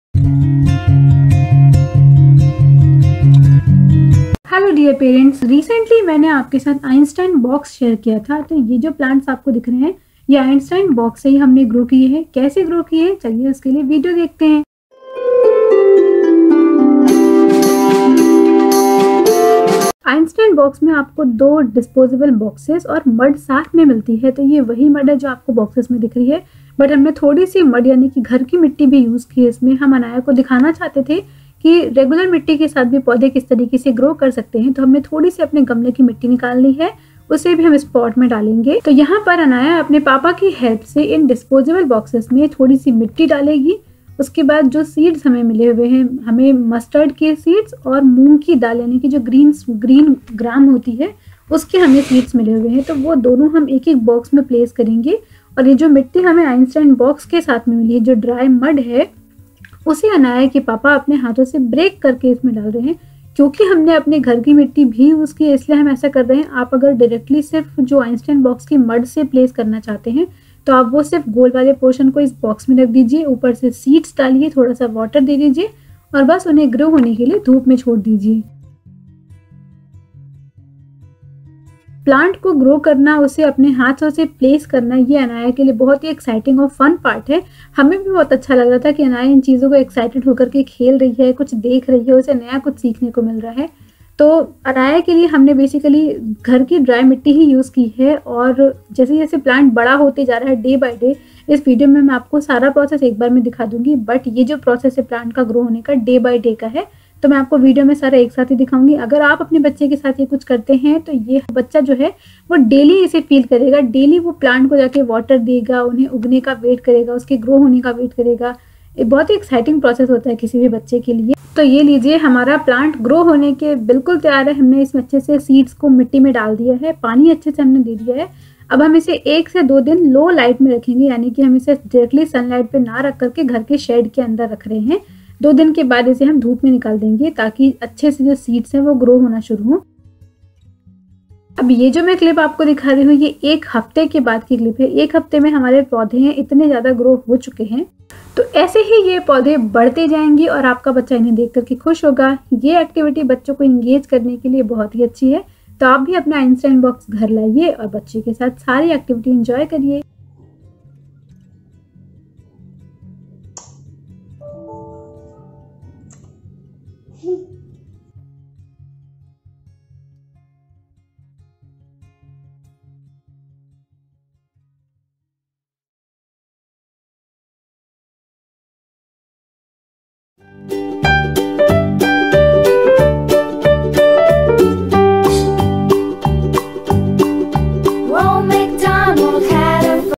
Anaya Grows a Plant | एक पौधा उगायें | Einstein Box Plant Growing Activity for Kids
Einstein box For 3-4 Year Kids | Unboxing and Review